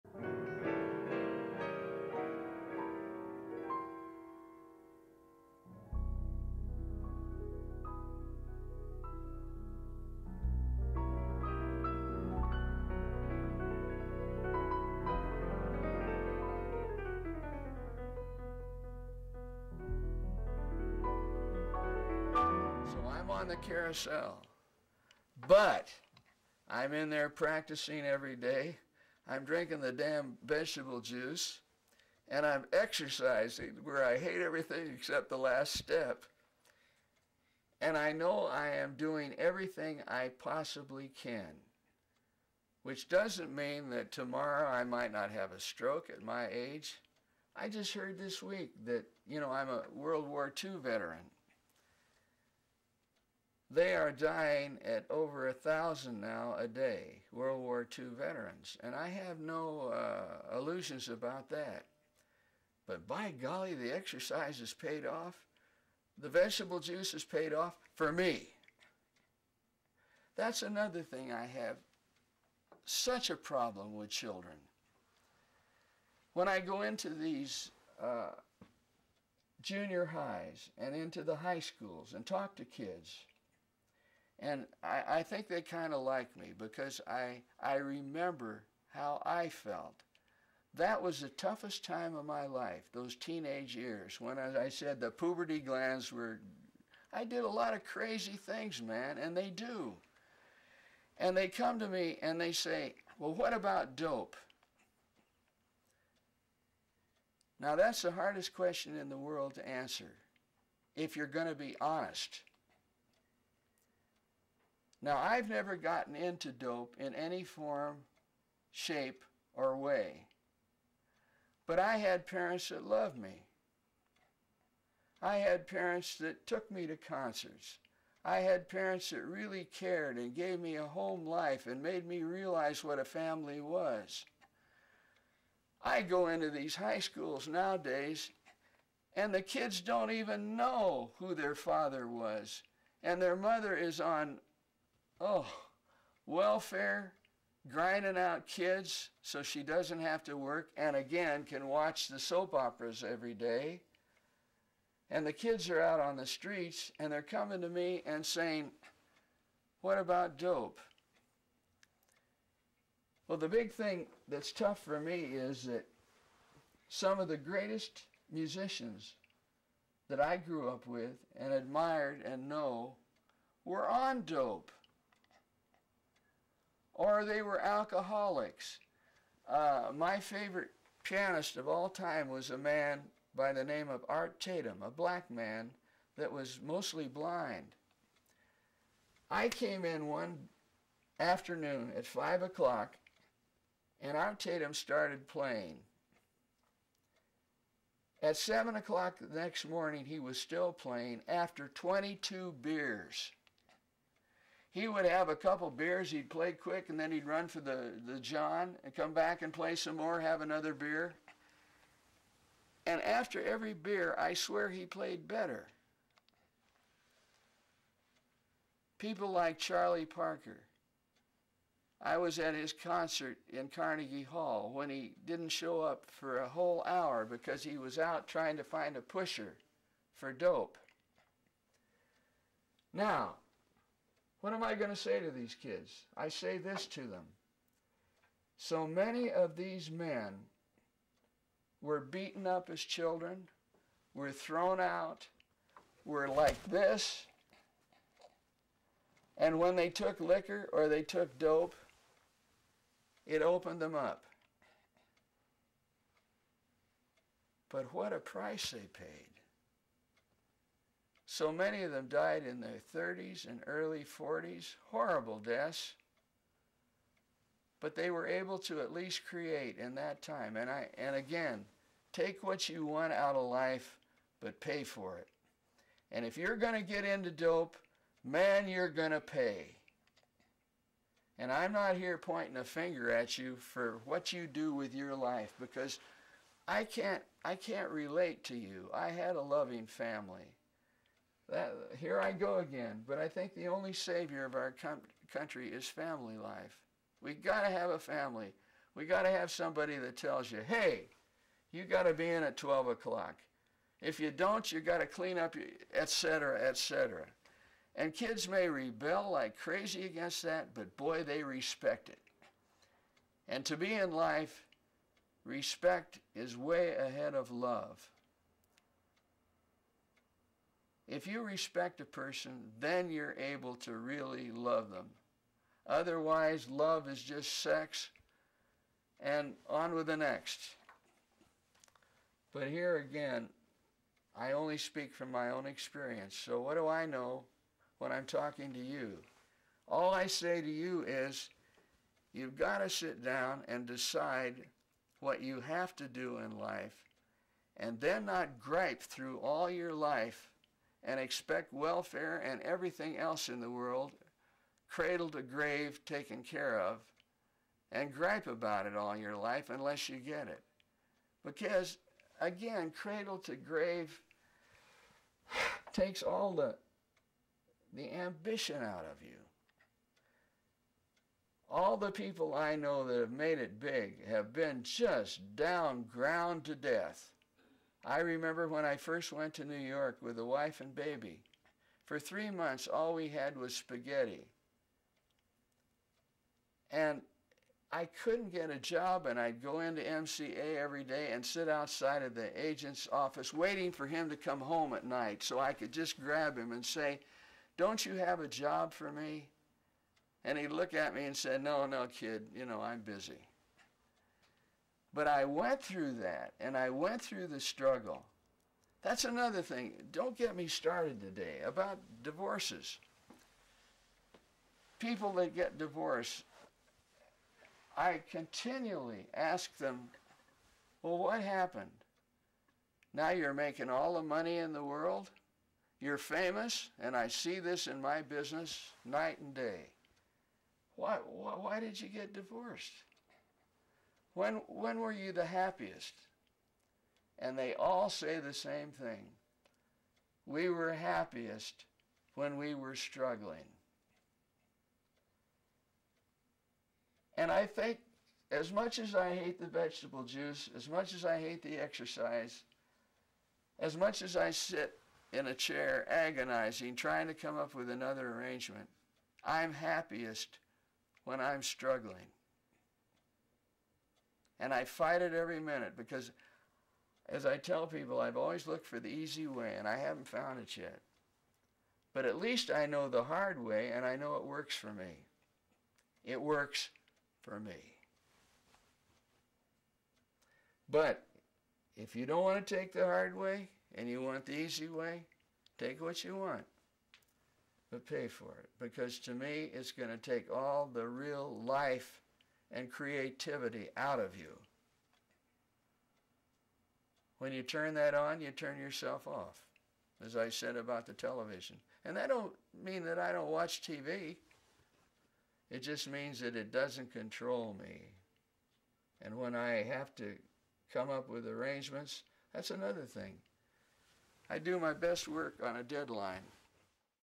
Roger Williams Up Close & Personal Part 4
Part 4 is another great part of the Up Close & Personal interviews with Roger. This great artist is a great human being that loves to reach out to other people.